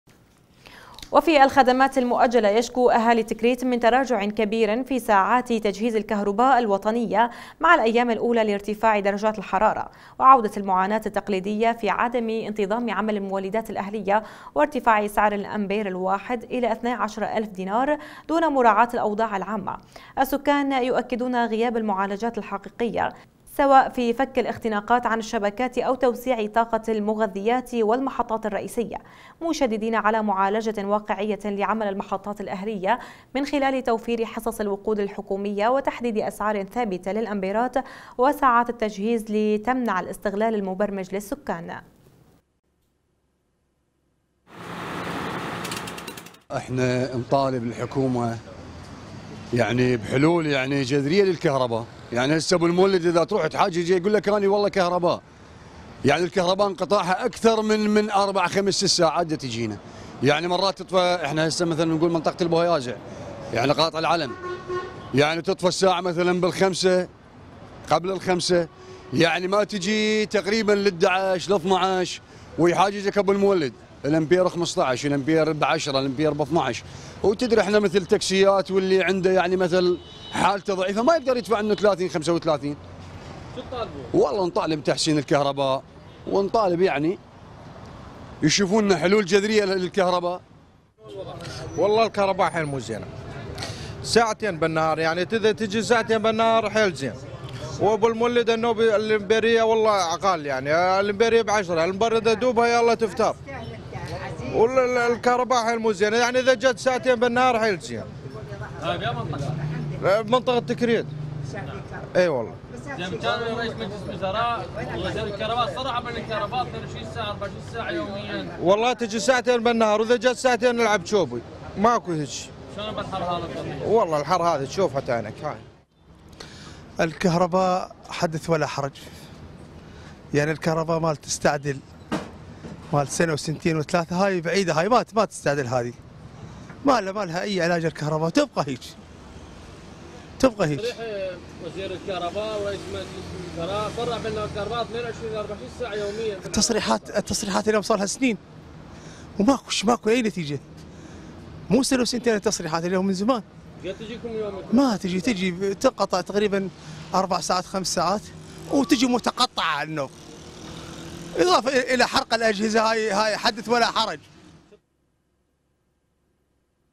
ساعات الكهرباء تزيد من لهيب الحر في تكريت
وفي الخدمات المؤجلة يشكو أهالي تكريت من تراجع  كبير في ساعات كبير في تجهيز الكهرباء الوطنية مع الأيام الأولى لارتفاع درجات الحرارة، وعودة المعاناة التقليدية في عدم انتظام عمل المولدات الأهلية و ارتفاع سعر الأمبير الواحد الى اثني عشر الف دينار دون مراعاة الأوضاع العامة، السكان يؤكدون غياب المعالجات الحقيقية سواء في فك الاختناقات عن الشبكات أو توسيع طاقة المغيذات و المحطات الرئيسية، مشديين على معالجة واقعية لعمل المحطات الأهلية من خلال توفير حصص الوقود الحكومية و تحديد أسعار ثابتة للأمبيرات و ساعات التجهيز لتمنع الاستغلال المبرمج للسكان.